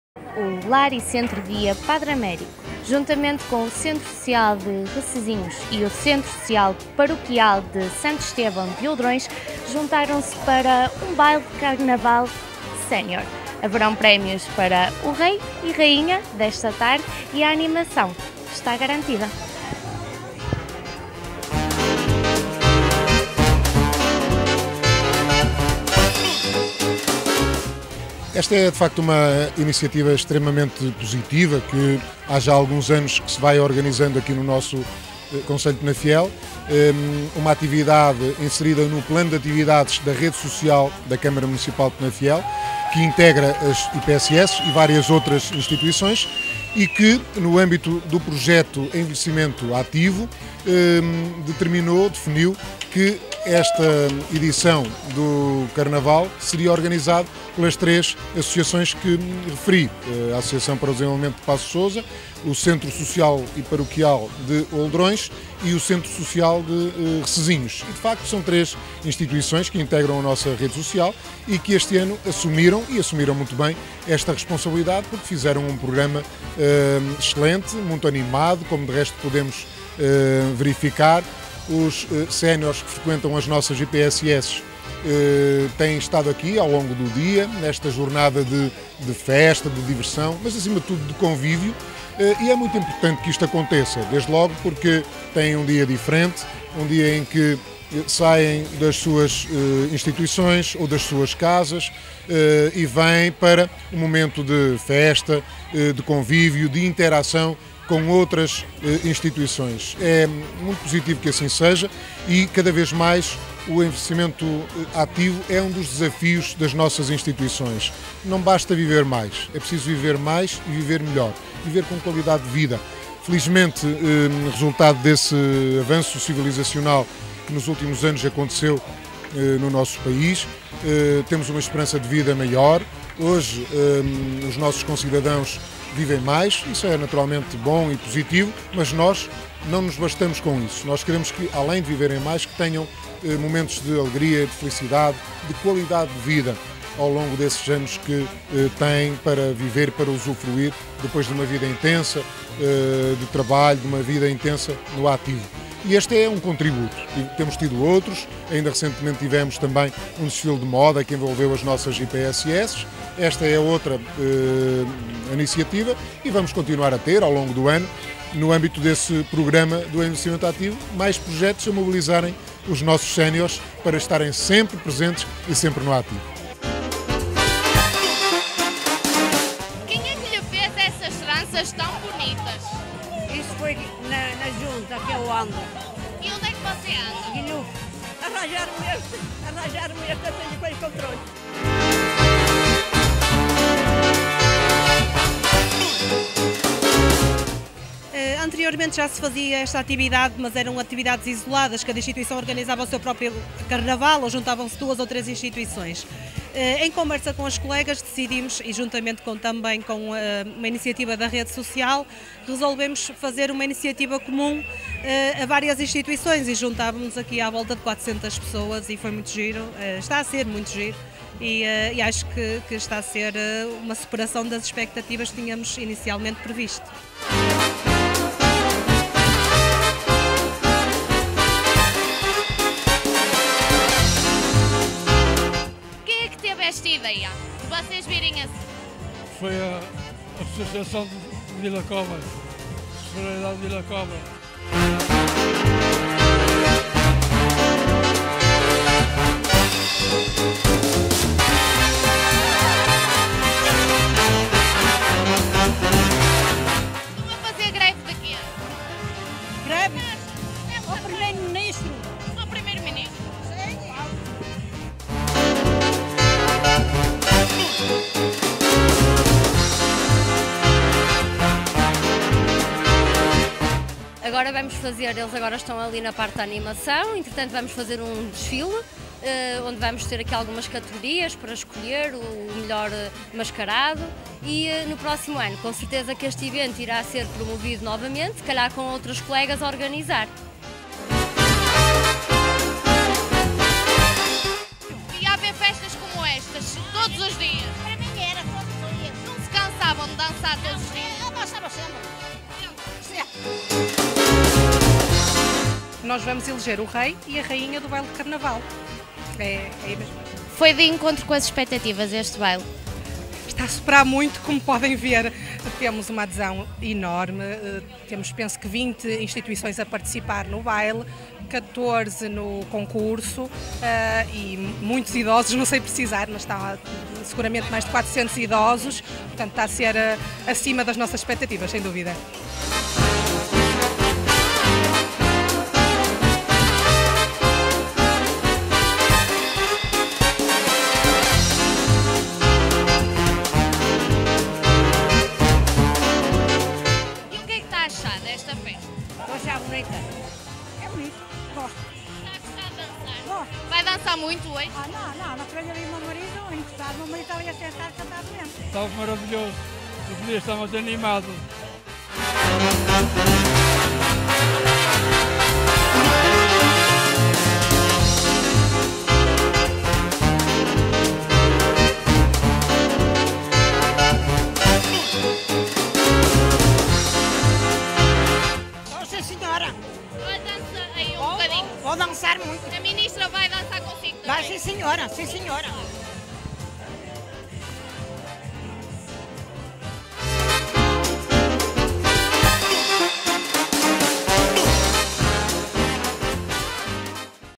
Carnaval Sénior em Penafiel
A Câmara Municipal de Penafiel, através da Rede Social, e no âmbito das atividades do "Envelhecimento Ativo", dinamizou na Quinta do Padrão, em Duas Igrejas, um Baile de Carnaval para os mais de 400 utentes séniores das 19 IPSS do concelho de Penafiel e dos Centros de Convívio da Junta de Freguesia de Penafiel.